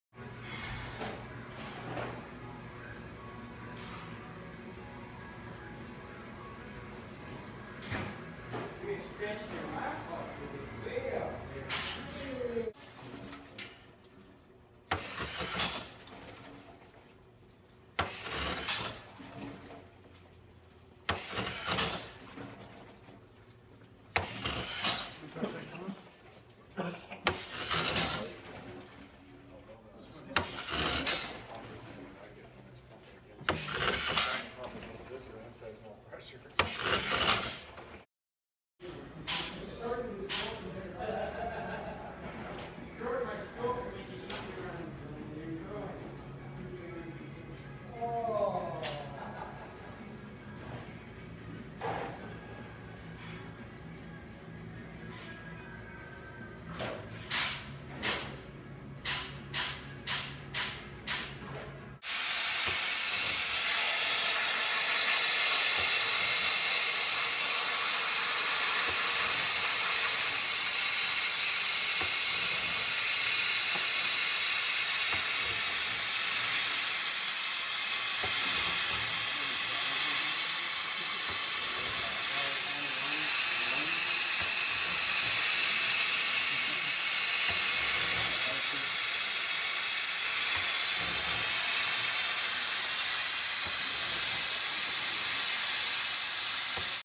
VP4 Pump ATEX | Pneumatic Landfill Environmental Pumps
The VP range is a cost effective, ATEX certified, reliable, automatic and long lasting solution to leachate, condensate and contaminated water control problems.